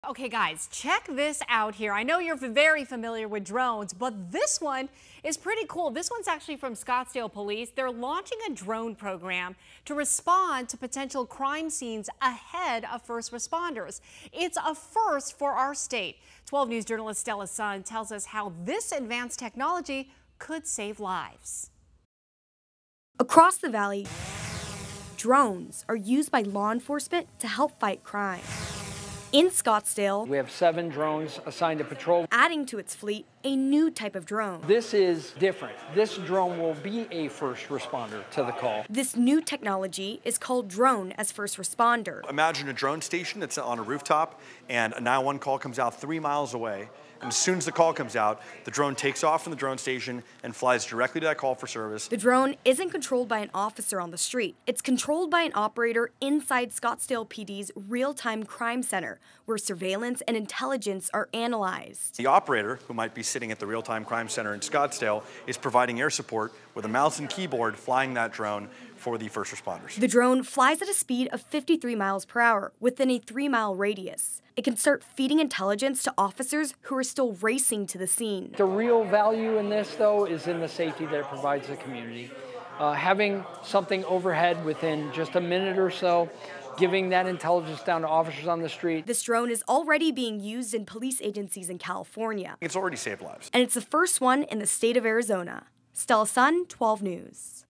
New drone program to act as 'first-responder' for Scottsdale PD
The new "drone-as-first-responder" can arrive to the scene of an emergency call within minutes